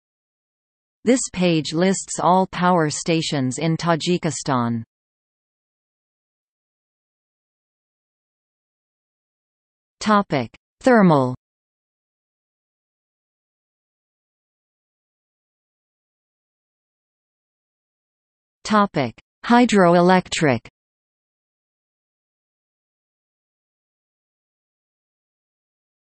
List of hydroelectric power stations in Tajikistan | Wikipedia audio article
00:00:06 Thermal
00:00:15 Hydroelectric

- Socrates

SUMMARY
This page lists all power stations in Tajikistan.